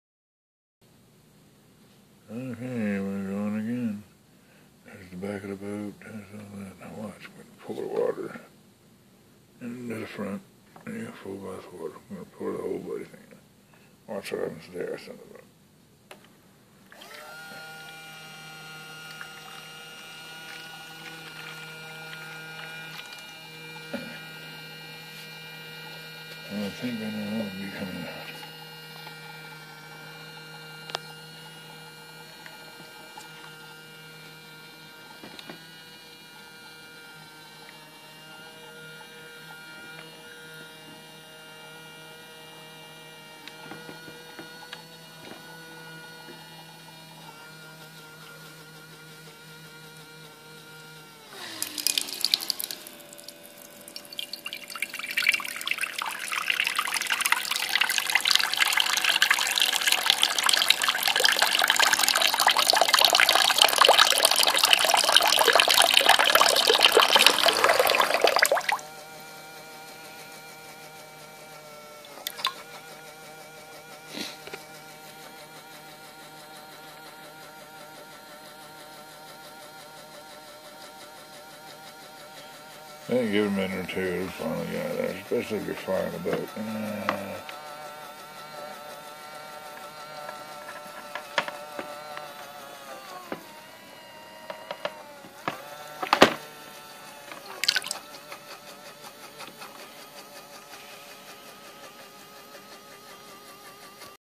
Bulge Water pump test. RC Boat
EDF airboat RC automatic bulge pump test.
Breadboard circuit.